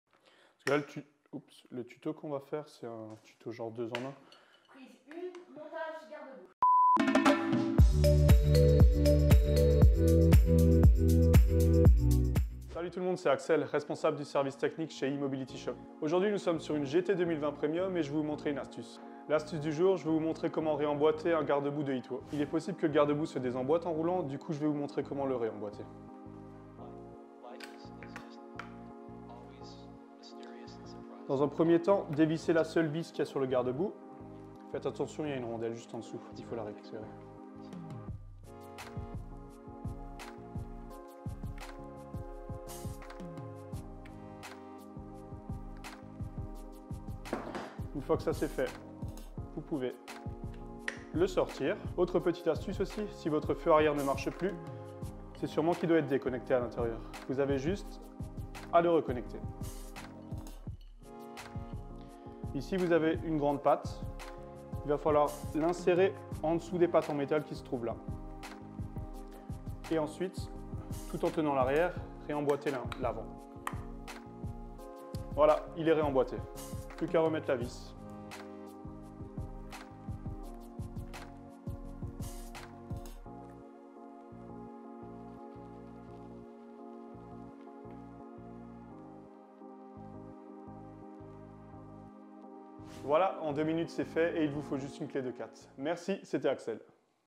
[TUTO] E-Twow : Changer un garde-boue GT 2020 Premium
Découvrez comment changer le garde boue d'une E-Twow GT2020 Premium

Matériel nécessaire :
- Une clé de 3